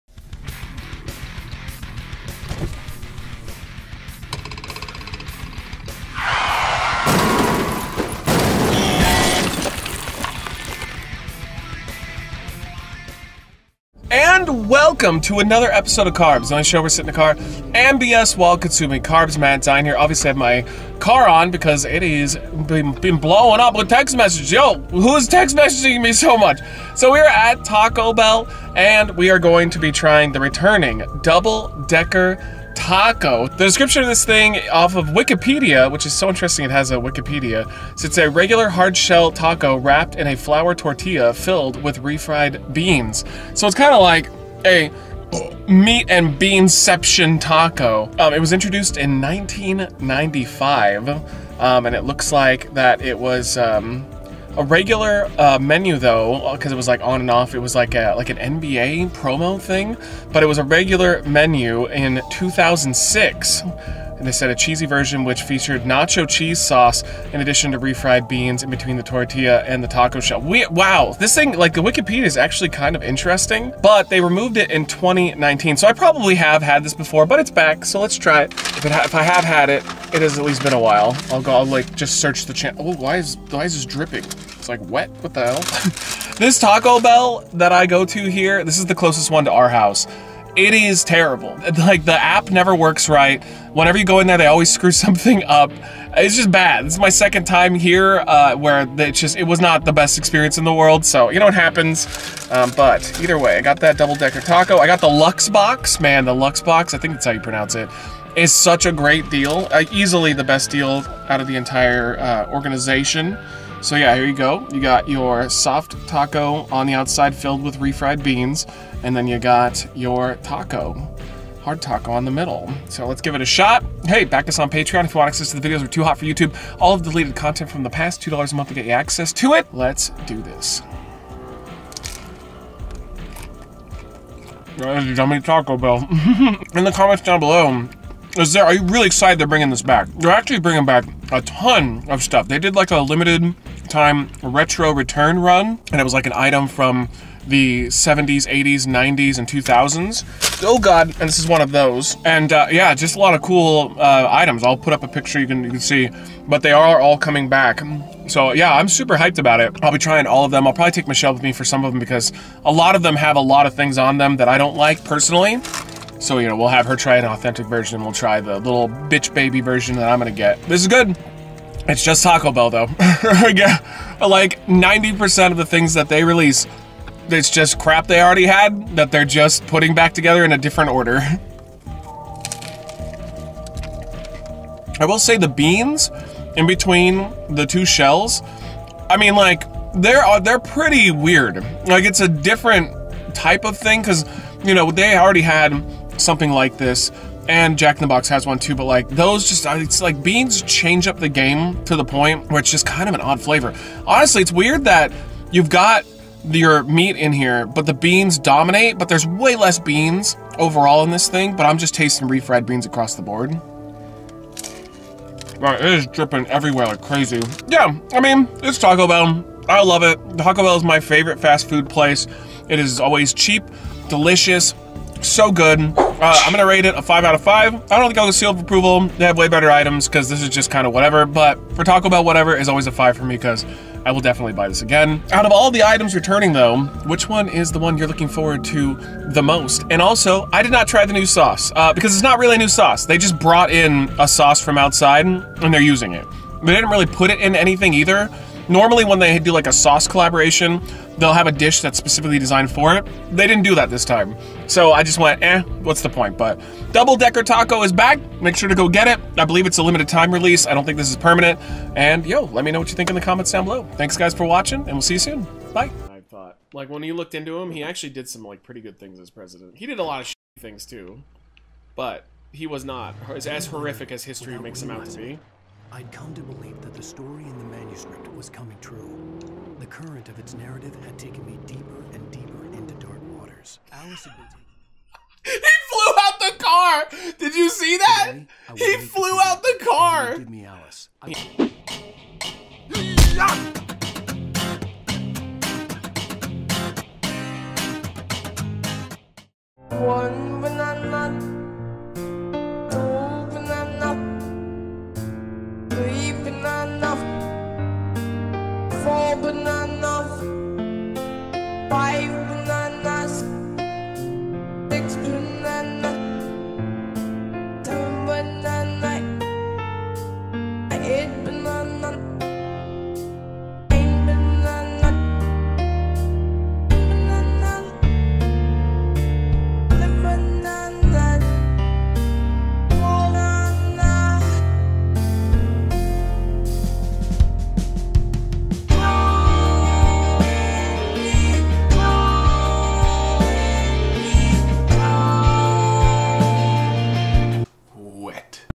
CarBS - Taco Bell Double Decker Taco Returns
On today's Wreckless Eating episode, Matt reviews the new the returning Taco Bell Double Decker Taco!